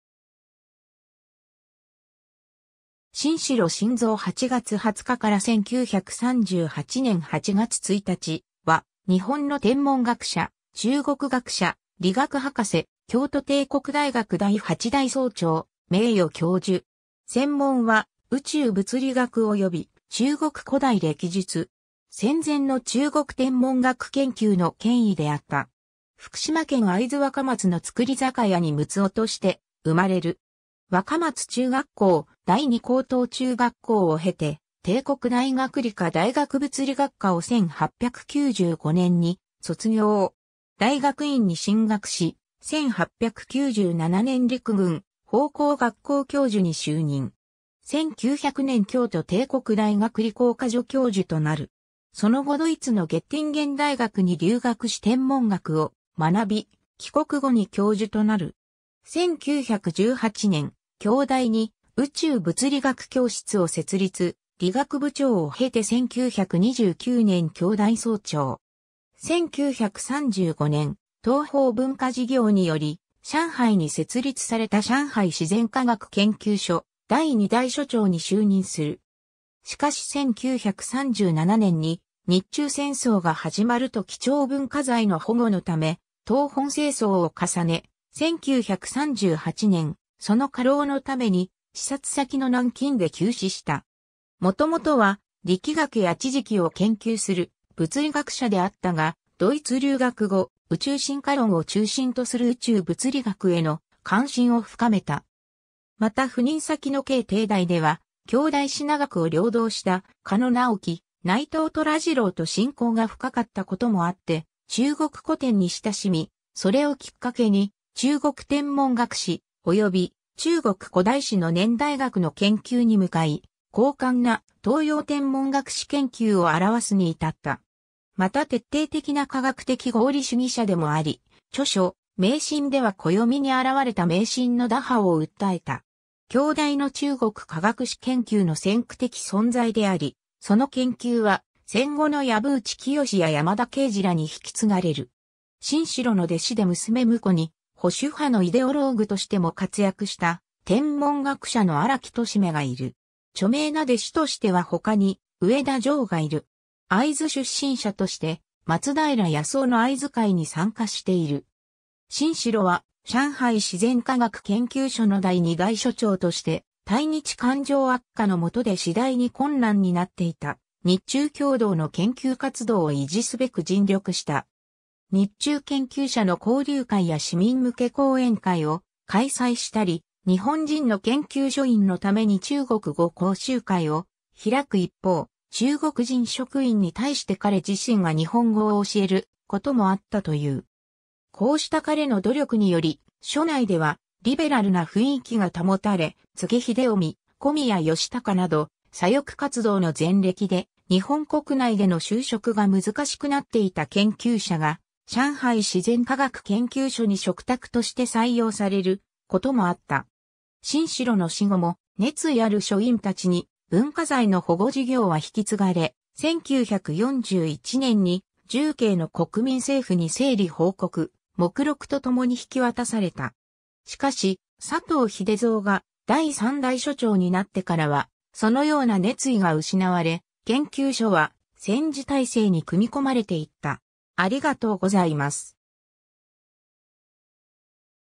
新城新蔵
新城 新蔵（しんじょう しんぞう、1873年（明治6年）8月20日 - 1938年（昭和13年）8月1日）は、日本の天文学者・中国学者、理学博士、京都帝国大学第8代総長・名誉教授。
専門は宇宙物理学および中国古代暦術。
戦前の中国天文学研究の権威であった。
 福島県会津若松の造り酒屋に六男として生まれる。
若松中学校（現福島県立安積高等学校）、第二高等中学校を経て帝国大学理科大学物理学科を1895年に卒業。
大学院に進学し、1897年陸軍砲工学校教授に就任。
 1900年京都帝国大学理工科助教授となる。
その後ドイツのゲッティンゲン大学に留学し天文学を学び、帰国後に教授となる。
1918年、京大に宇宙物理学教室を設立、理学部長を経て1929年京大総長。
 1935年、東方文化事業により上海に設立された上海自然科学研究所第2代所長に就任する。
しかし1937年に日中戦争が始まると貴重文化財の保護のため東奔西走を重ね、1938年、その過労のために視察先の南京で急死した。
 もともとは力学や地磁気を研究する物理学者であったが、ドイツ留学後、宇宙進化論を中心とする宇宙物理学への関心を深めた。
また赴任先の京帝大では京大支那学を領導した狩野直喜・内藤虎次郎と親交が深かったこともあって中国古典に親しみ、それをきっかけに中国天文学史、および中国古代史の年代学（天文考古学）の研究に向かい、浩瀚な『東洋天文学史研究』（序文は内藤が執筆）を著すに至った。
また徹底的な科学的合理主義者でもあり、著書『迷信』では暦に現れた迷信の打破を訴えた。
 京大の中国科学史研究の先駆的存在であり、その研究は戦後の藪内清や山田慶児らに引き継がれる。
 新城の弟子で娘婿に、保守派のイデオローグとしても活躍した天文学者の荒木俊馬がいる。
著名な弟子としてはほかに上田穣がいる。
 会津出身者として、松平保男の会津会に参加している。
 新城は上海自然科学研究所の第2代所長として、対日感情悪化のもとで次第に困難になっていた日中共同の研究活動を維持すべく尽力した。
日中研究者の交流会や市民向け講演会を開催したり、日本人の研究所員のために中国語講習会を開く一方、中国人職員に対して彼自身が日本語を教えることもあったという。
こうした彼の努力により所内ではリベラルな雰囲気が保たれ、柘植秀臣・小宮義孝など左翼活動の前歴で日本国内での就職が難しくなっていた研究者が上海自然科学研究所に嘱託として採用されることもあった。
 新城の死後も、熱意ある所員たちに文化財の保護事業は引き継がれ、1941年に重慶の国民政府に整理報告・目録とともに引き渡された。
しかし、佐藤秀三が第3代所長になってからはそのような熱意が失われ、研究所は戦時体制に組み込まれていった。